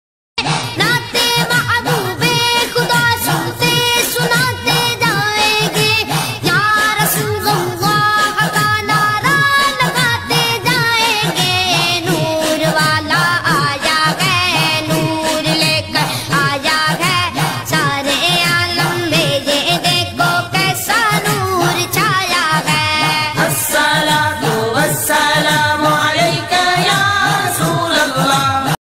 JULOOS E MOHAMMADI ❤🇮🇳❤30/10/2020 EID MILAD UN NABI MUBARAK HO AAP SABHI KO ❤ 🇮🇳 ❤
JULOOS E MOHAMMADI ❤🇮🇳30/10/2020

EID MILAD UN NABI MUBARAK HO AAP SABHI KO ❤ 🇮🇳 ❤

Under : MUSLIM YUVA MANCH & MITHAPUR MODERN CLUB

MITHAPUR, DALKHOLA, UTTAR DINAJPUR,  WEST BENGAL